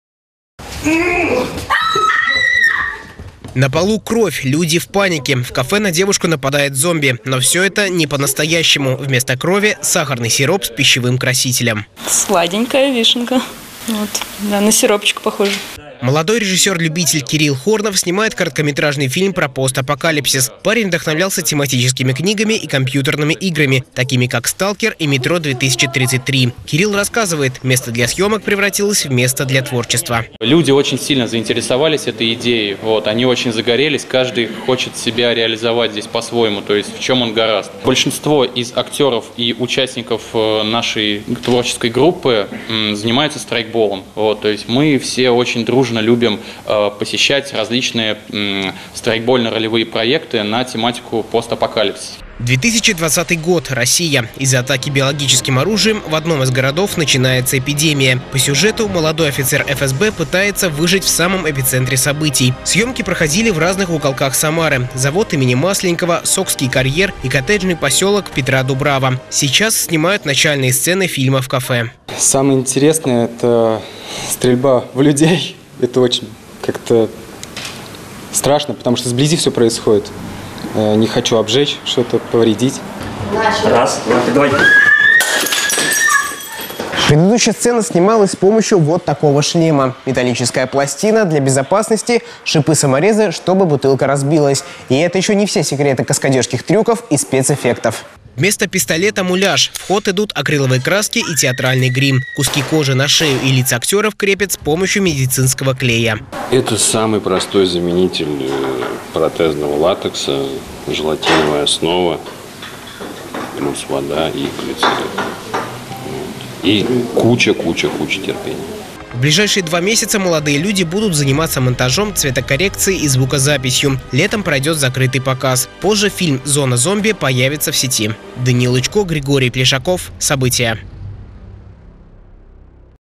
Самарские режиссеры сняли короткометражный триллер
Зомби атакуют самарцев.  В городе бушует биологический вирус.   Все  это, по счастью, исключительно  фантазии молодых актеров  и режиссеров, что снимают сейчас фильм  о постапокалипсисе. Триллер хоть и малобюджетный, но полностью укладывается в каноны жанра. Бутафорская кровь, желатин, имитирующие человеческую плоть и  и пистолеты-муляжи. Даниил Лучко  продемонстрировал крепкие нервы.